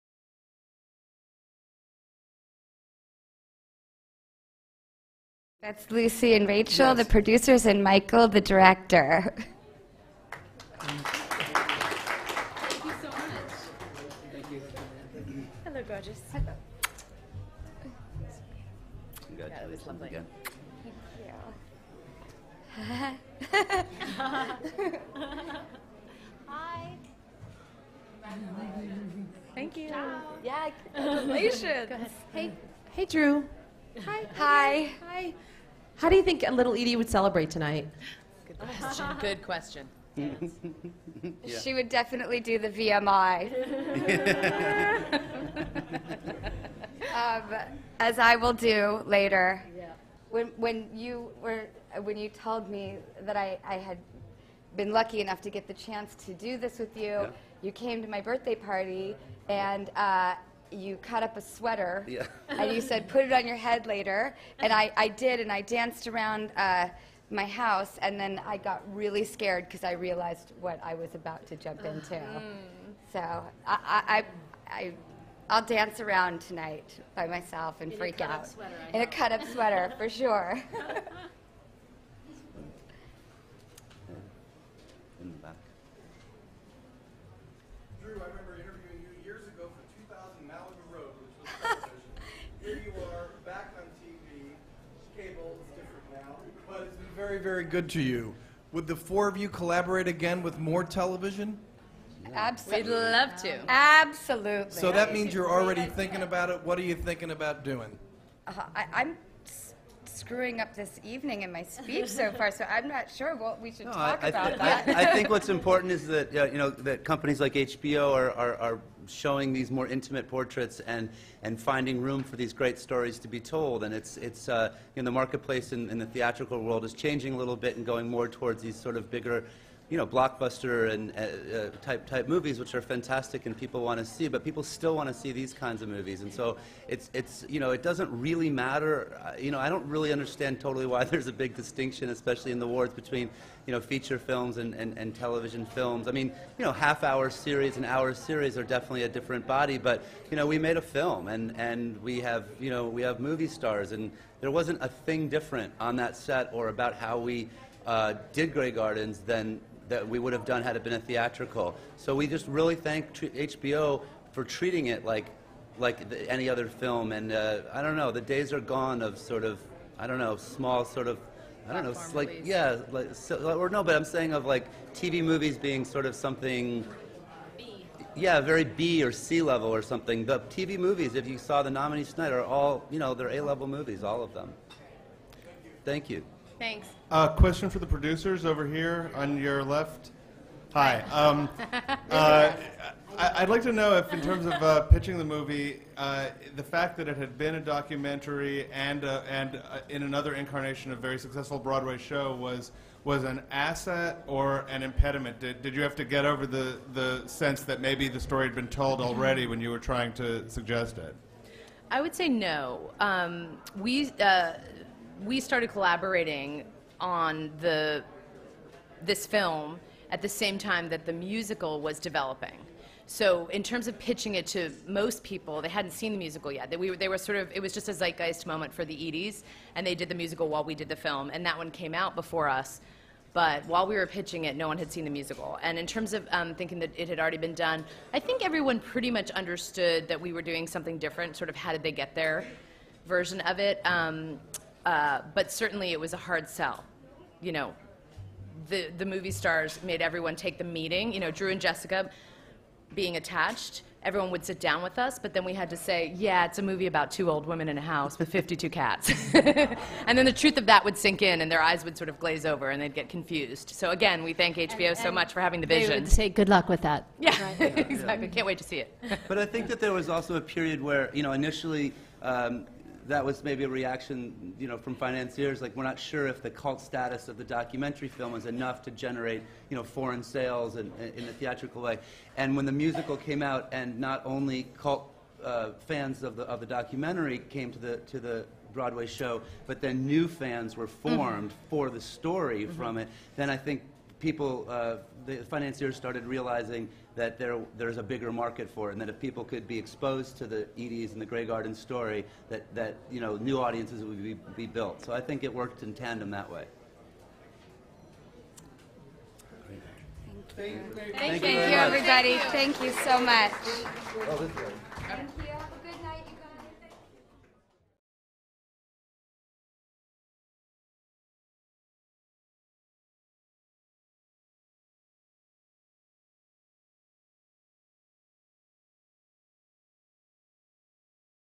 Grey Gardens Wins 2010 Golden Globes
the personal blog of Jerry Torre who appeared in the original 1975 Grey Gardens documentary by the Maysles brothers.

The producers of HBO's Grey Gardens answer questions after winning the 2010 Golden Globes for Best TV Movie